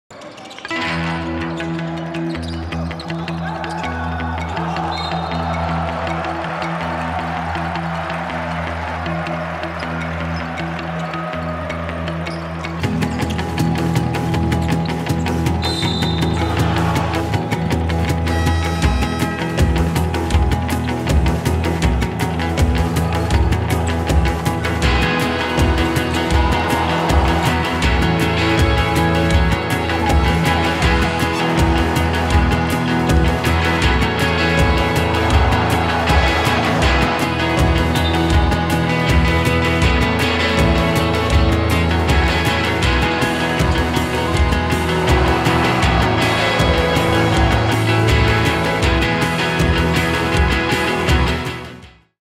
Top 5 plays of Cibona in the 2018/19 season
We present you the best 5 plays of Cibona from the 2018/19 ABALiga season.